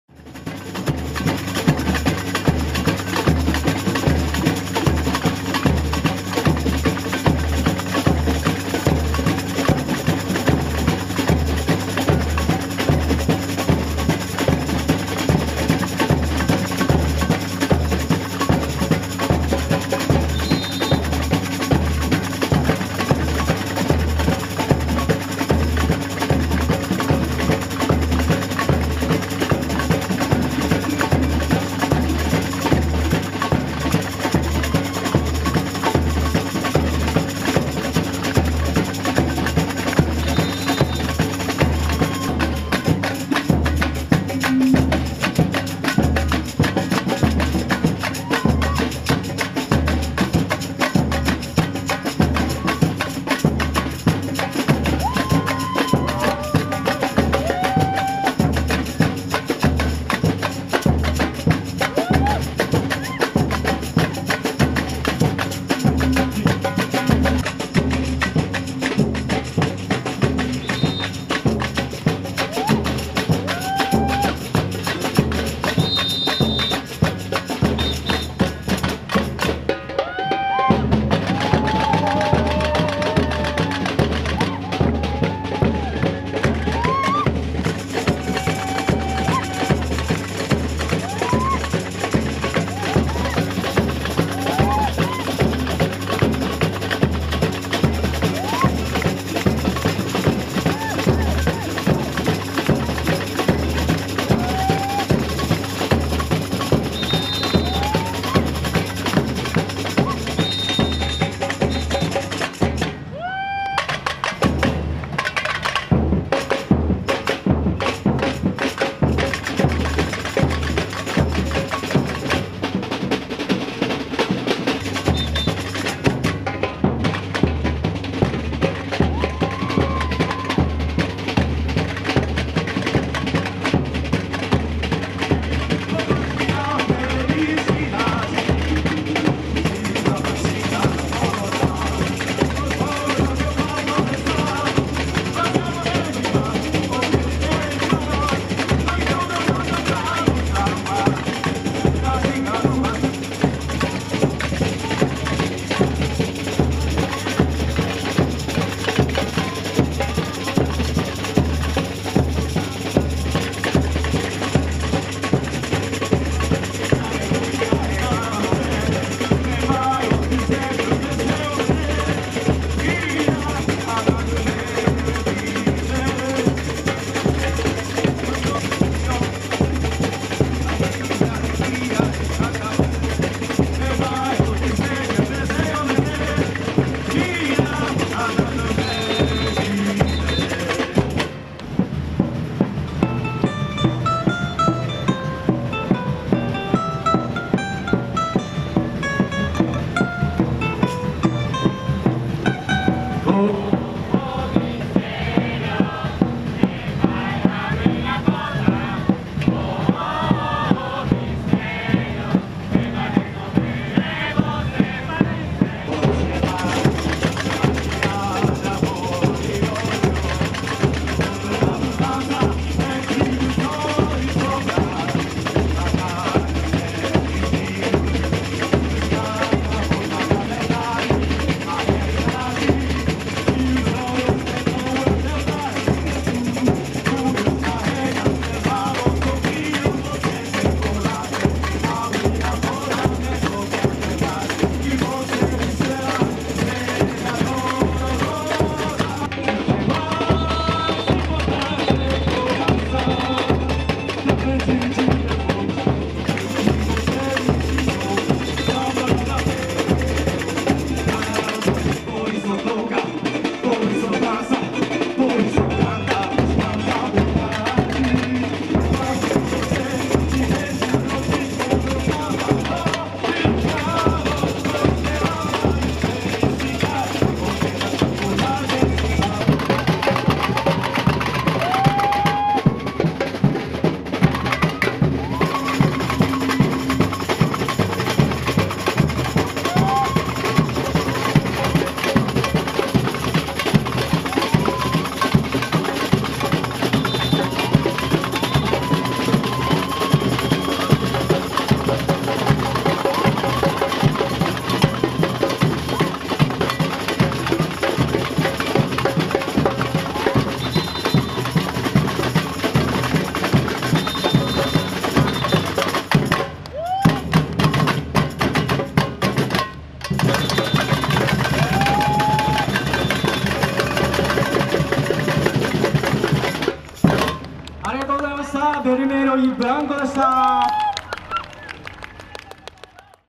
情熱の草加よさこいサンバ vol.2 ヴェルメーリョ・イ・ブランコ Dynamic SAMBA in SOKA 2017
4K60P　情熱の草加よさこいサンバ vol.2
Dynamic SAMBA in SOKA-YOSAKOI FESTIVAL 2017 vol.2
Performer: G.R.C.E.S. VERMELHO E BRANCO ヴェルメーリョ・イ・ブランコ

情熱の草加よさこいサンバ vol.1
リベルダージ